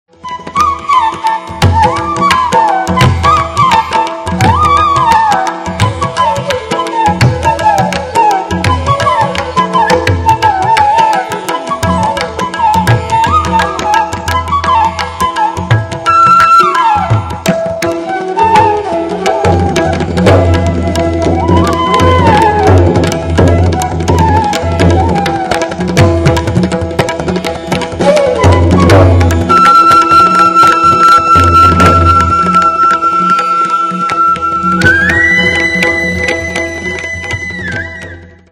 Enchanting with Morning Ragas | Classical Music
Enjoy Morning Ragas from the archives of All India Radio & Doordarshan.